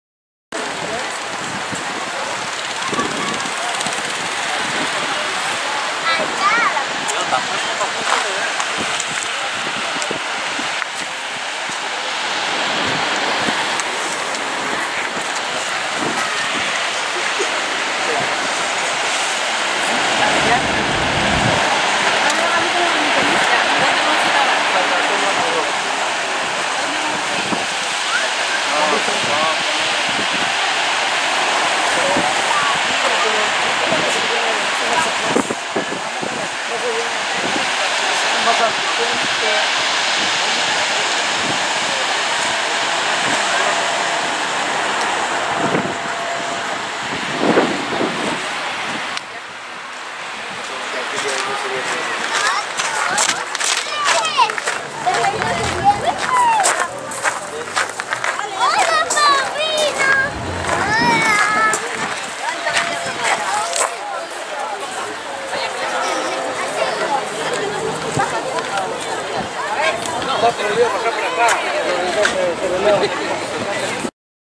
Paseando por el Parque del Amor en Miraflores--Costa Verde
Un recorrido por esta parte del distrito de Miraflores  hoy  25  de diciembre 2015.